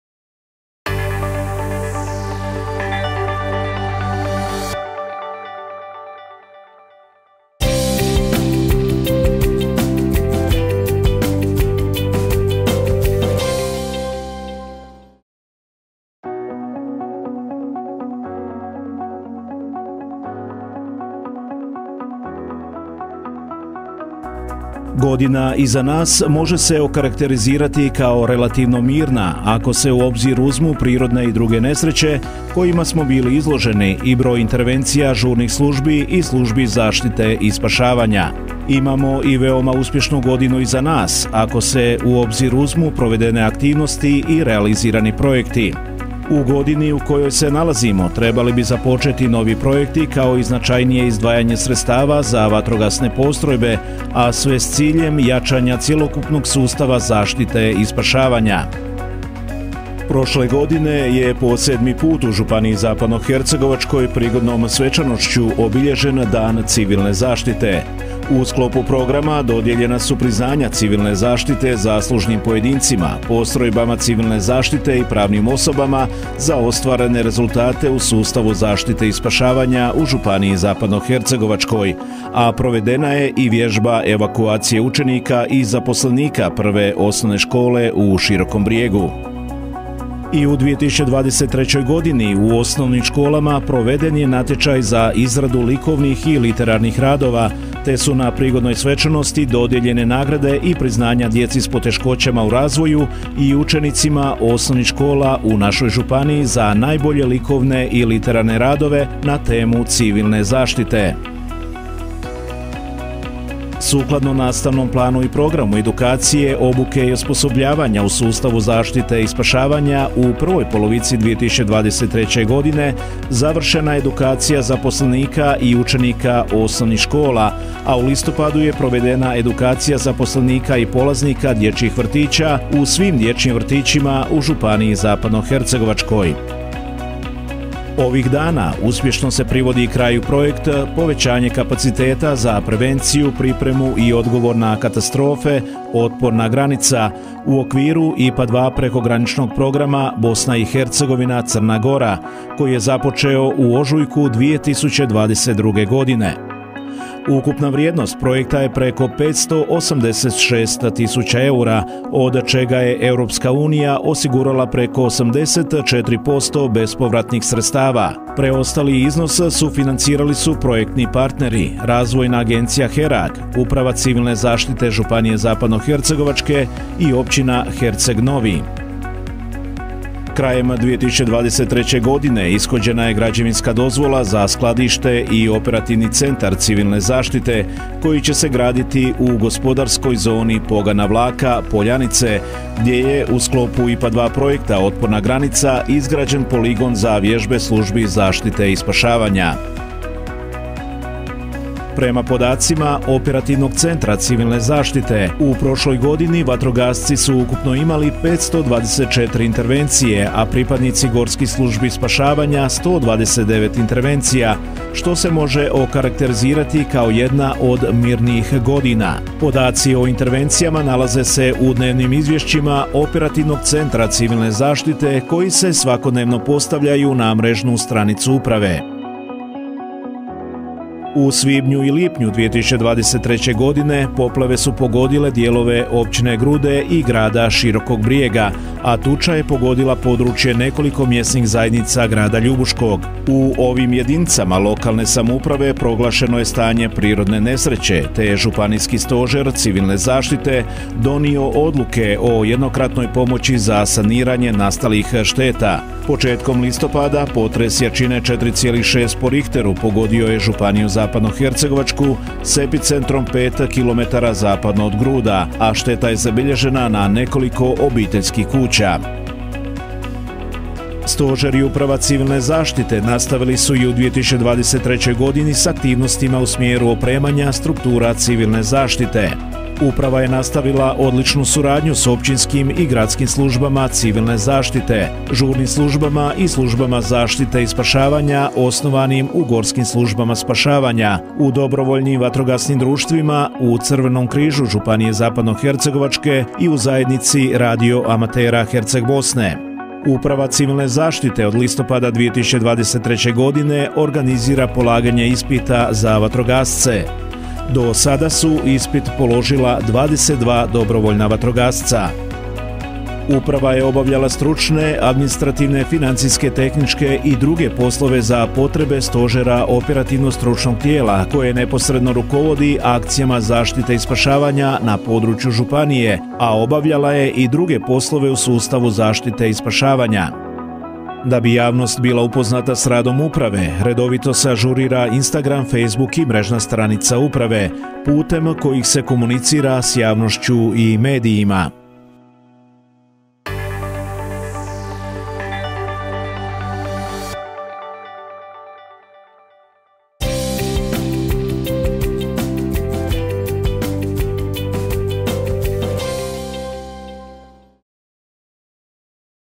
Aktivnosti Uprave civilne zaštite Županije Zapadnohercegovačke u 2023. godini
U organizaciji Uprave civilne zaštite Županije Zapadnohercegovačke i Županijskog stožera civilne zaštite u Posušju je, 27. veljače 2024. godine, svečanom sjednicom Županijskog stožera civilne zaštite obilježen Međunarodni dan civilne zaštite i Dan civilne zaštite Županije Zapadnohercegovačke. U sklopu obilježavanja Dana civilne zaštite prikazan je ovaj 6-minutni video o aktivnostima Uprave civilne zaštite Županije Zapadnohercegovačke u 2023. godini.